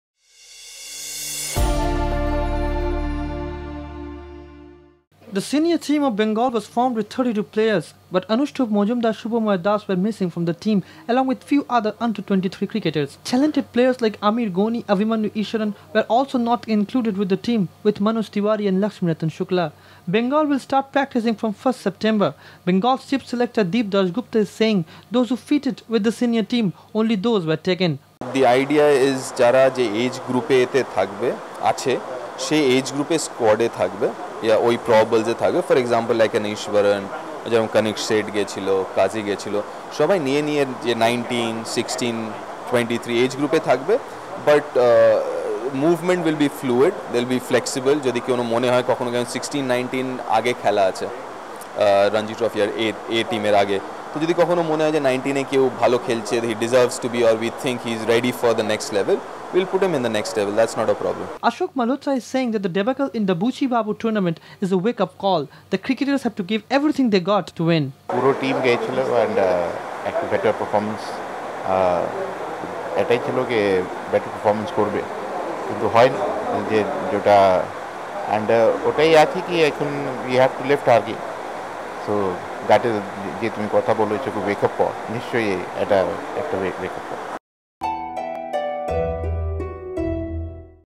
Anustup,Subhamoy face Selectors' axe !
The probable Senior team of Bengal was formed with 32 players,but Anustup Majumdar,Subhamoy Das were axed along with others.Lets hear what Bengal Cricket's chief selector Deep Dasgupta is saying.